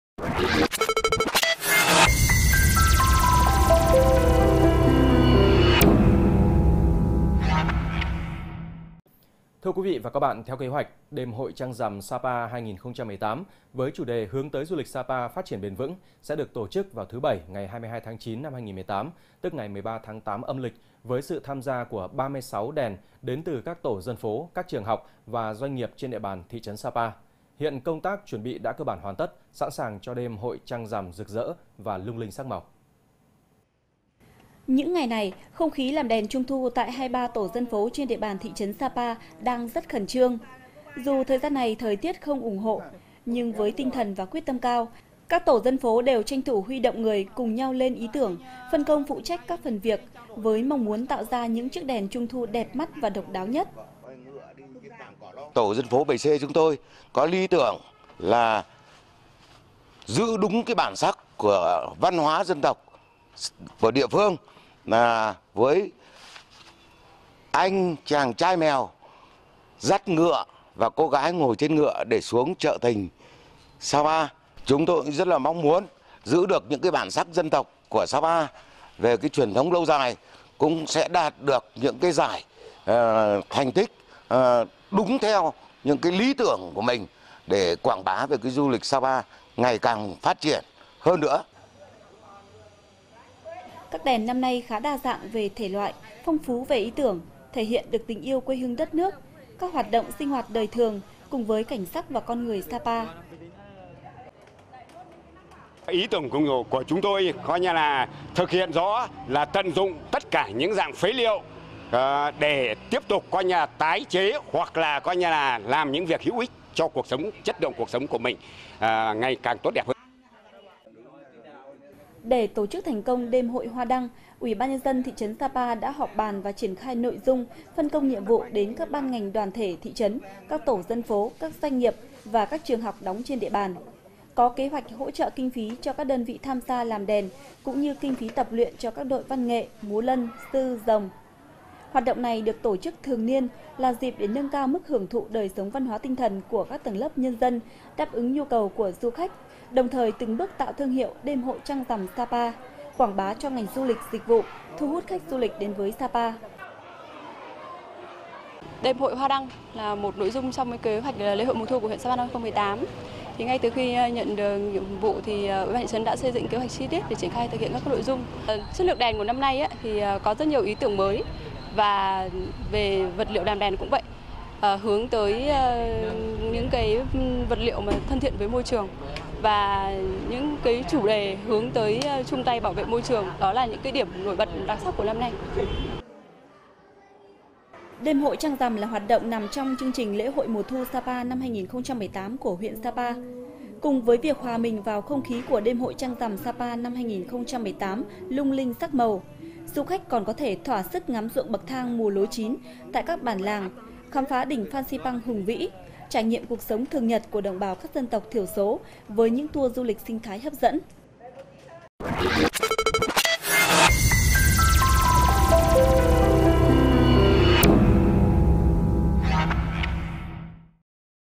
Sa Pa sẵn sàng cho "Đêm hội trăng rằm 2018" | THLC
Laocaitv.vn - Theo kế hoạch, “Đêm hội trăng rằm Sa Pa 2018” với chủ đề “Hướng tới Du lịch Sa Pa phát triển bền vững” sẽ được tổ chức vào thứ Bảy, ngày 22/9/2018, tức ngày 13/8 âm lịch, với sự tham gia của 36 đèn đến từ các tổ dân phố, các trường học và doanh nghiệp trên địa bàn thị trấn Sa Pa. Hiện, công tác chuẩn bị đã cơ bản hoàn tất, sẵn sàng cho đêm hội trăng rằm rực rỡ và lung linh sắc màu.

Đài Phát thanh - Truyền hình Lào Cai